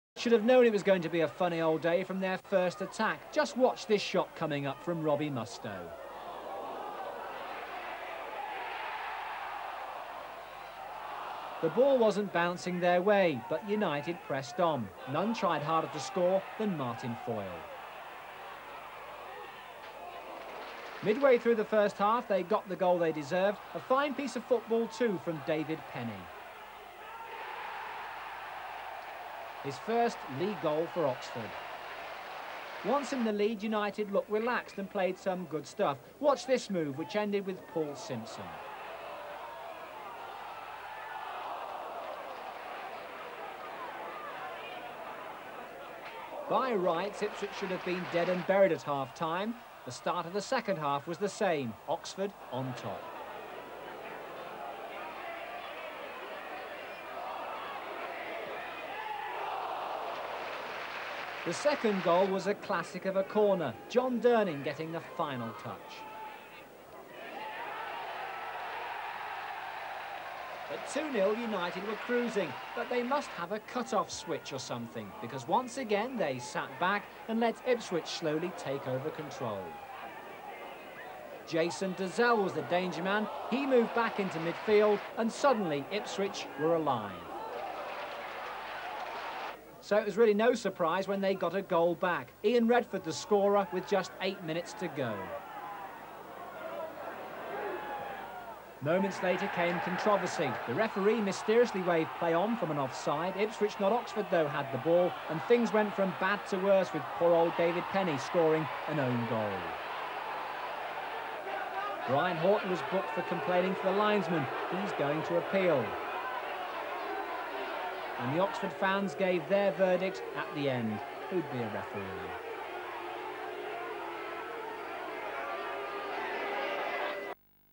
Oxford United 2 Ipswich Town 2 23rd September 1989 Football League Division Two
An entertaining match from the Manor, with Oxford taking a 2-0 lead through Dave Penney and John Durnin.  Ipswich fight back for a point, with an Ian Redford goal and a Penney own goal.